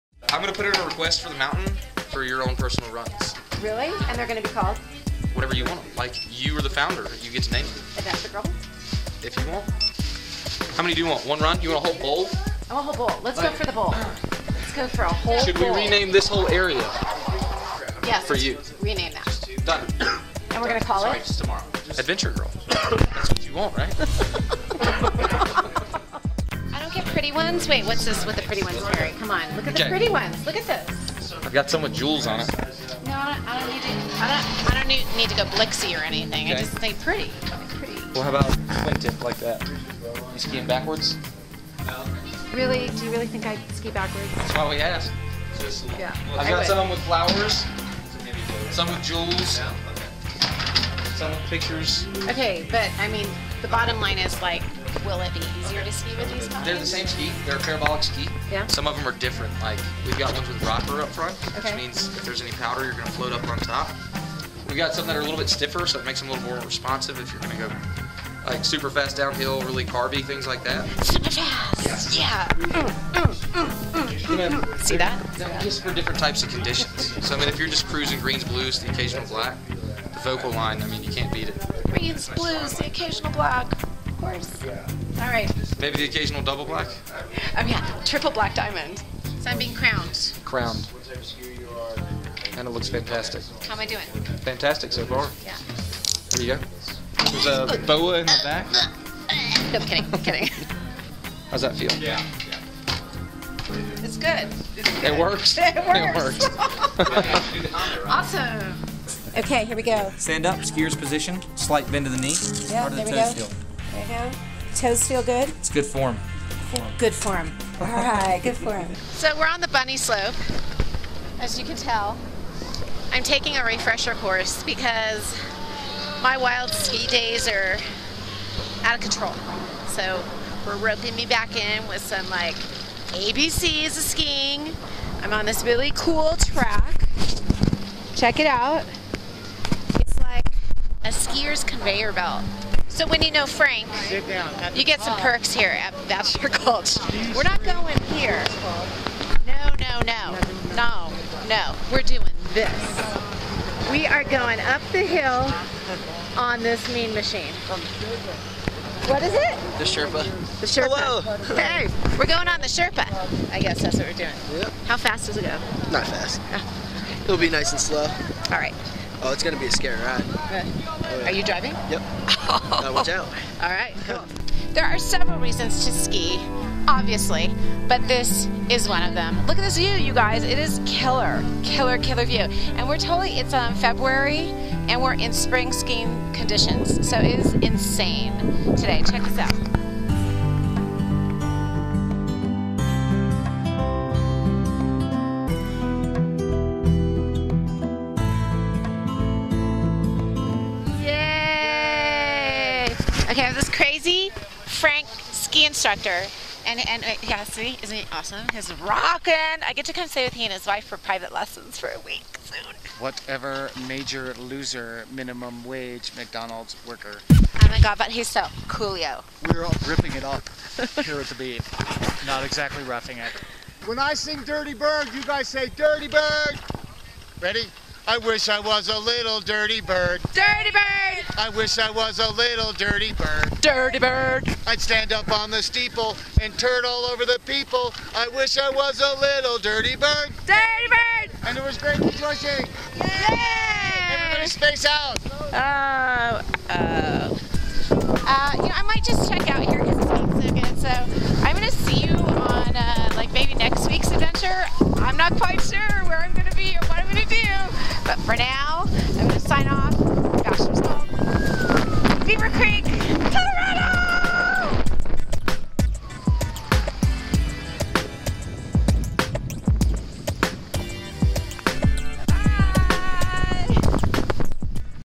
Adventure Girl Bachelor Gulch, Colorado Ski Adventure
Adventure Girl, Stefanie Michaels hits the slopes for this week's adventure in Colorado. From the ski shop, the bunny slope and a dirty bird parody, Stef brings you behind-the-scenes hilarity on the slopes of Bachelor Gulch.

JumpshipGold